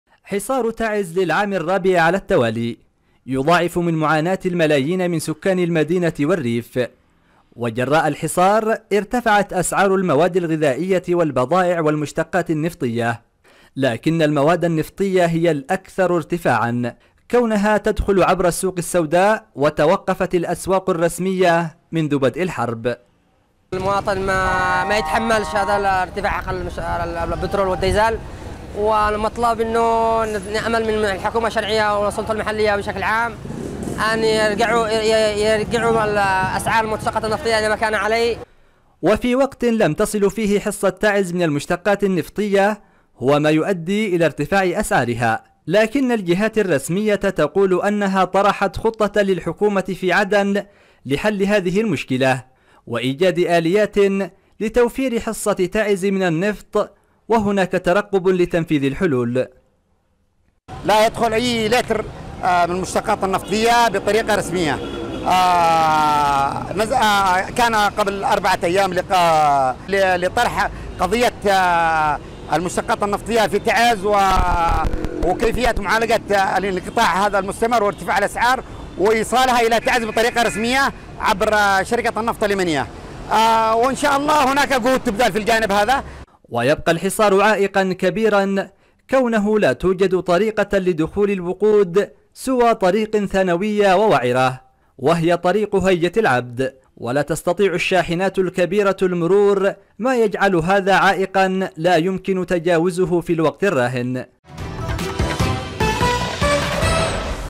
تعز.. ارتفاع اسعار المشتقات النفطية وسبل المعالجة
التردد الجديد للقناة على مدار النايلسات (11603 افقي H ) ارتفعت أسعار المشتقات النفطية نسبيا خصوصا في تعز، في وقت يقف فيه الحصار على تعز عائقا أمام دخول شاحنات النفط الكبيرة، وما تزال تعز تنتظر حصتها الرسمية من الوقود بعد تقديم رؤيتها للحل إلى الحكومة ..